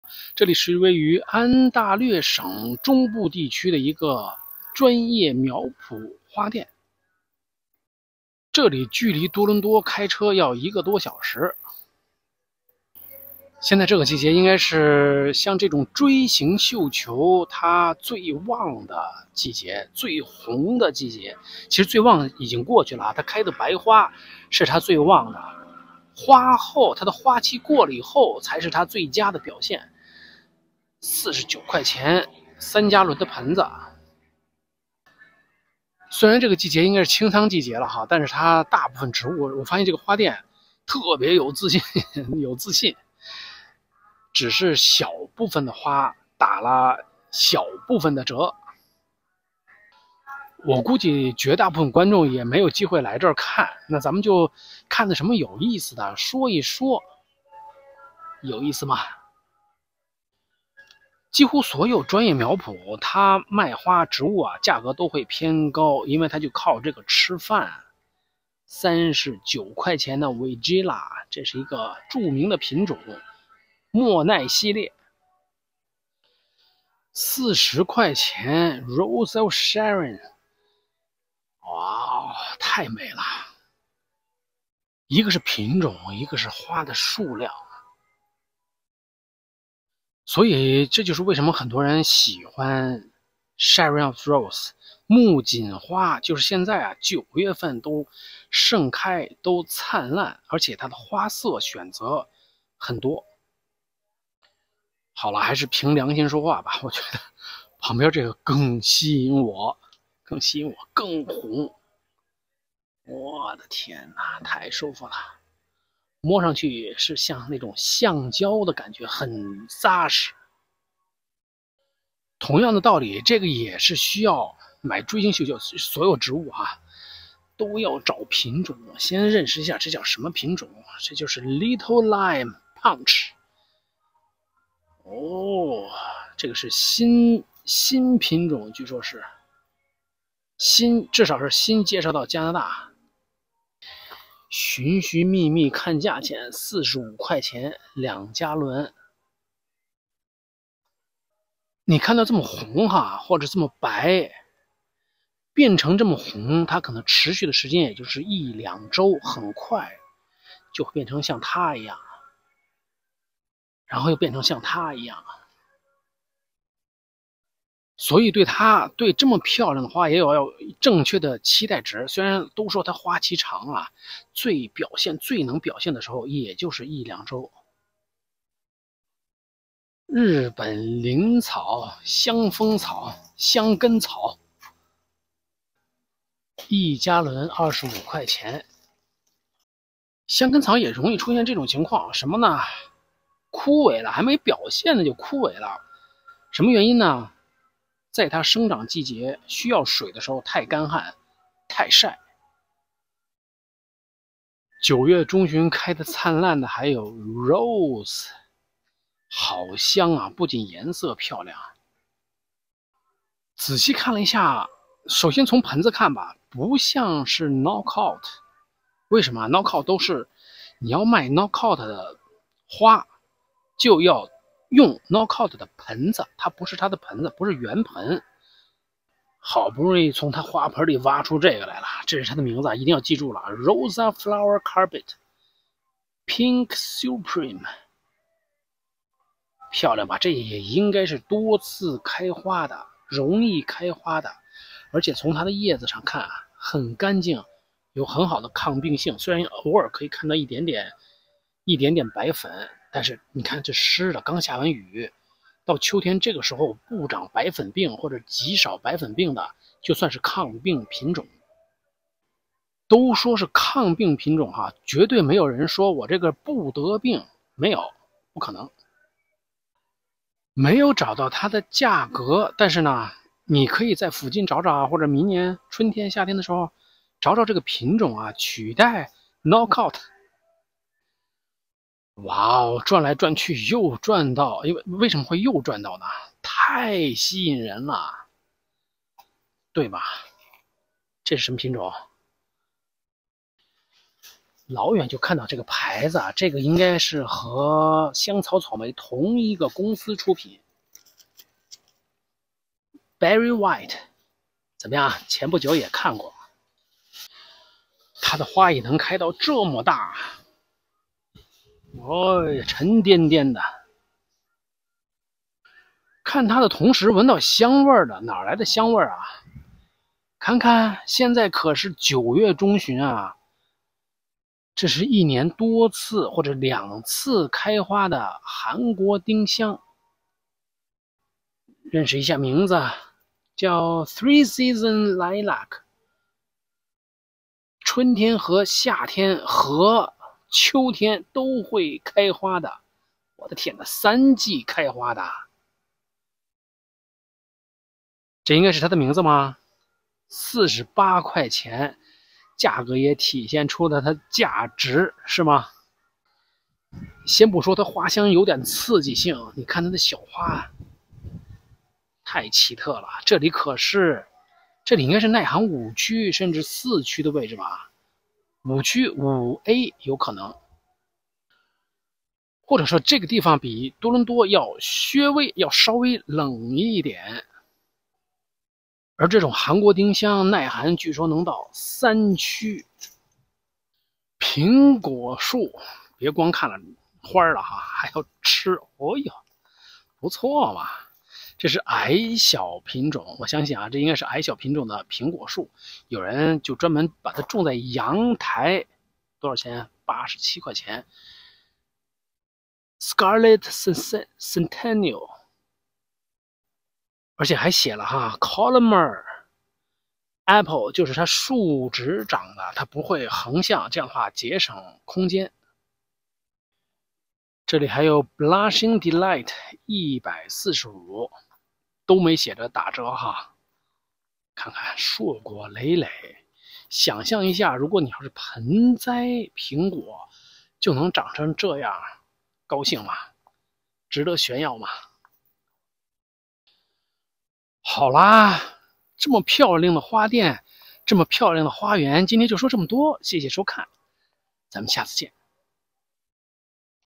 秋天的花店，欣赏火红的绣球、三季开花的韩国丁香、盛开的红月季、盆栽苹果树
地址：Grobe Nursery & Garden Centre
1787 Greenhouse Rd, Breslau, ON N0B 1M0

Google搜索：石针+花名，看看有没有你需要的视频和文章